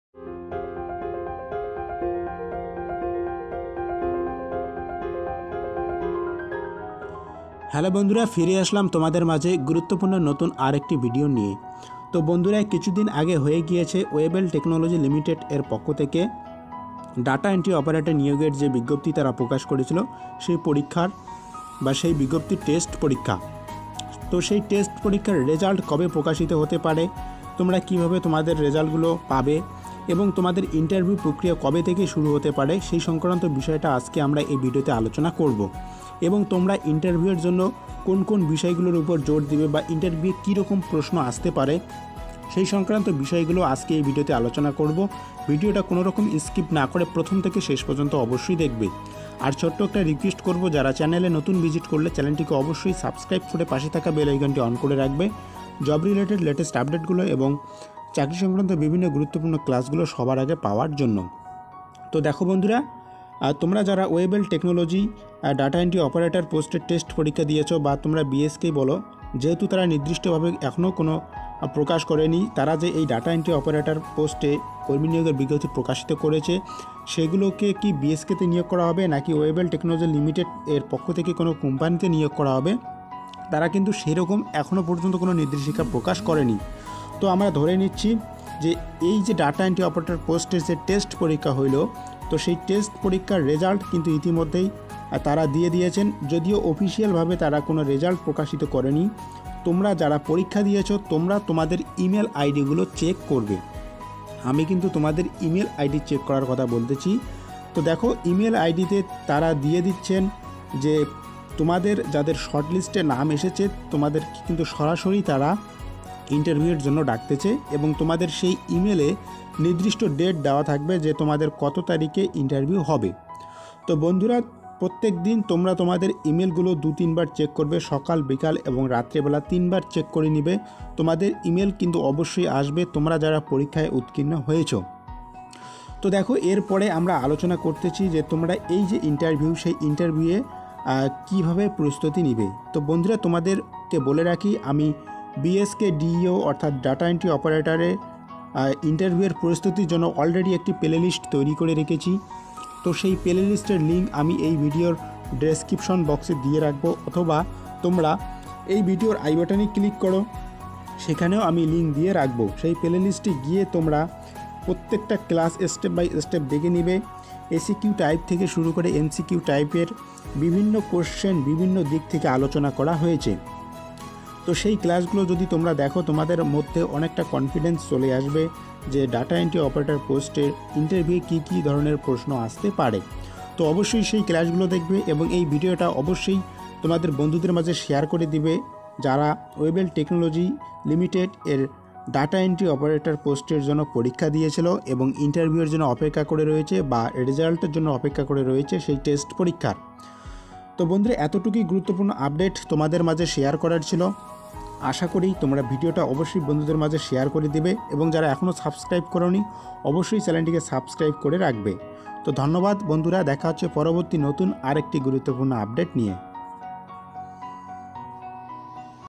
BSK/WTL-DEO shortlist published 2022 || Bsk update today|| Bsk result update
বাংলা সাহায়তা কেন্দ্র নিয়ে কিছু আপডেট এবং রেজাল্ট সম্পর্কিত কোন মতামত থাকলে কমেন্ট বক্সে অবশ্যই জানাবেন।

তোমাদের আপকামিং বিভিন্ন এক্সামের জন্য চ্যালেনে অনেক প্রাকটিস সেট করানো হয়েছে সেগুলো অবশ্যই দেখে নিও